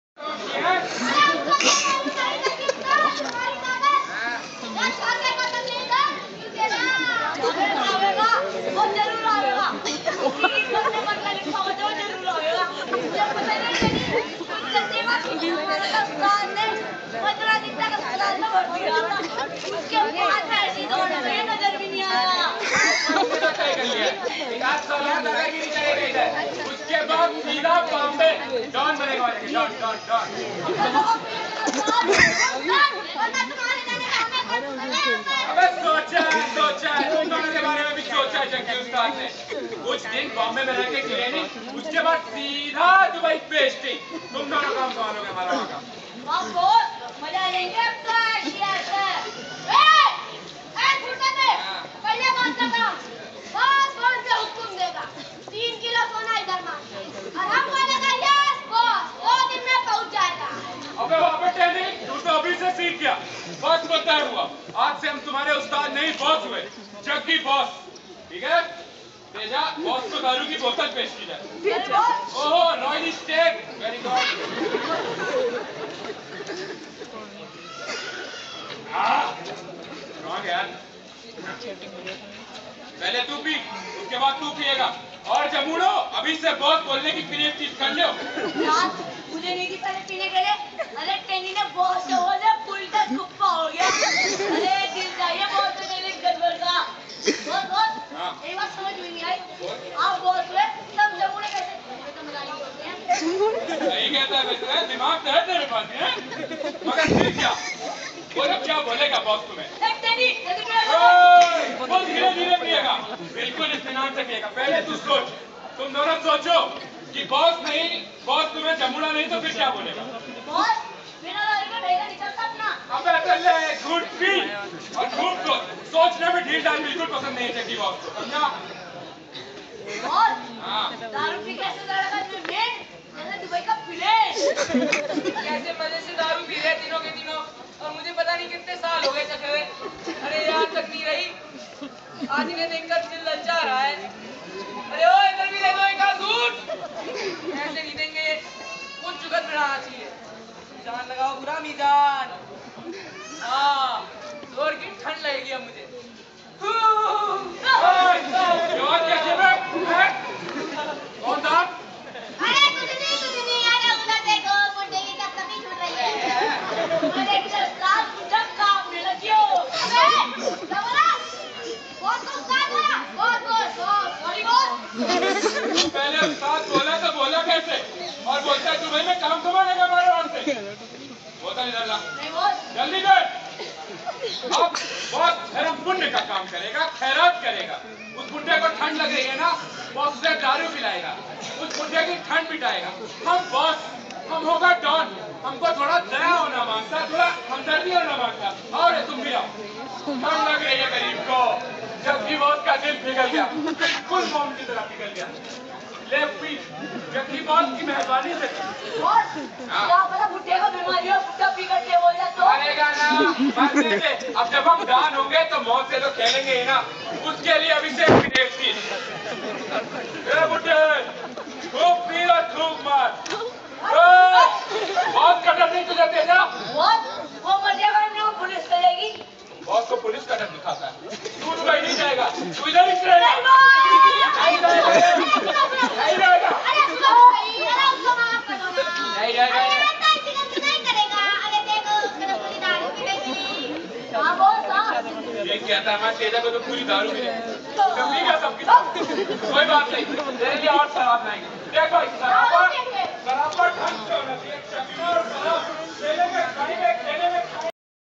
Qadir's Act. At Col. Brown school
Qadir hashim Acted in Drama, in Col. Brown cambridge school.